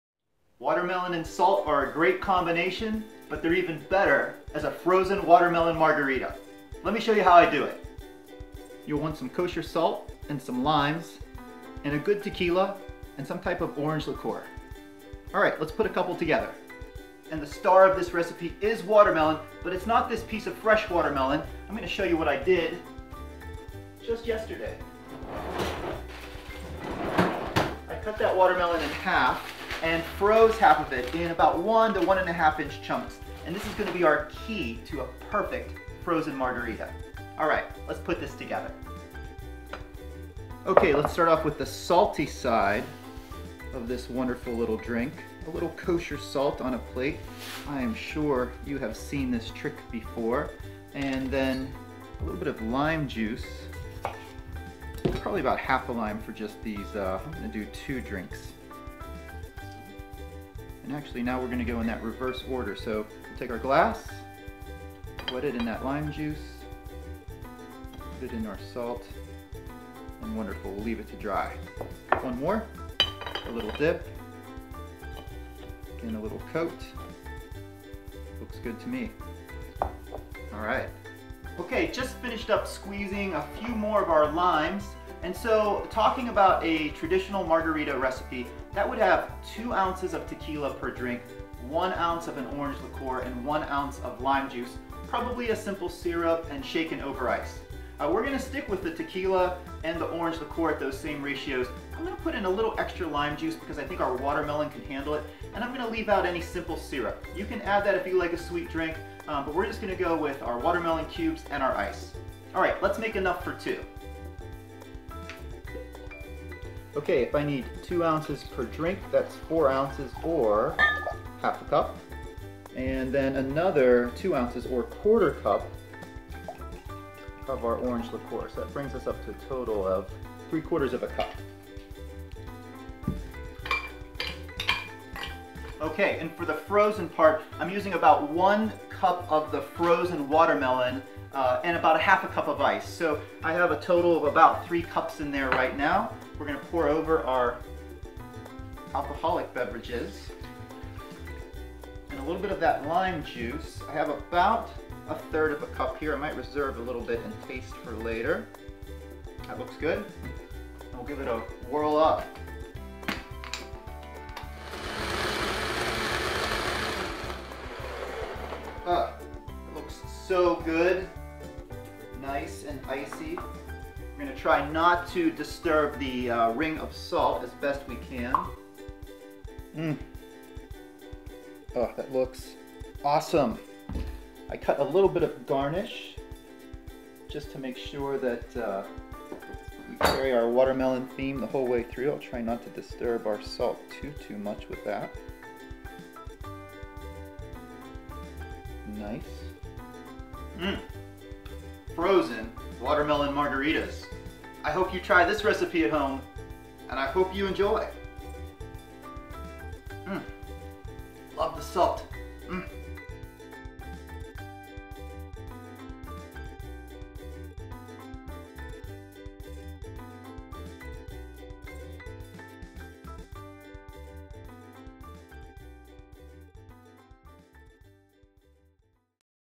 Frozen Watermelon Margaritas | Doug Cooking
Frozen Watermelon Margaritas! Here is how to make frozen watermelon margarita cocktails at home! This watermelon margarita recipe is easy and refreshing with some planning and a blender. Good clear tequila and orange liquor spike up this high-quality cool libation. The pairing fresh lime juice, watermelon, and salt reaches new heights in this frozen drink that symbolizes summer, but you may be making these margaritas all year long. Marg on!

Kosher Salt
Limes: https
Fresh Whole Watermelon

Recipe recap:

1 cup Frozen Watermelon Cubes, 1 inch dice
1/2 cup Ice Cubes
2 ounces Tequila
1 ounce Orange Liqueur
1-2 ounces Fresh Squeezed Lime Juice

Additional Limes for Garnish
Kosher Salt
Optional Slices of Fresh Watermelon for Garnish

Recipe recap:

Prepare serving glasses by dipping rim of glass in a saucer of lime juice, then a saucer of kosher salt. Set aside.

Add remaining ingredients to blender and pulse until combined. Pour into salt-rimmed glass and garnish with melon or lime wedges. Enjoy!

0:00 - Intro
0:09 - Ingredients
0:40 - Salt the glass rim
1:26 - Review of traditional vs watermelon margarita
1:56 - Measure the spirits
2:15 - Measure watermelon and blend
2:54 - Pour the drinks and garnish
3:33 - Recipe recap

All Production by Doug

2016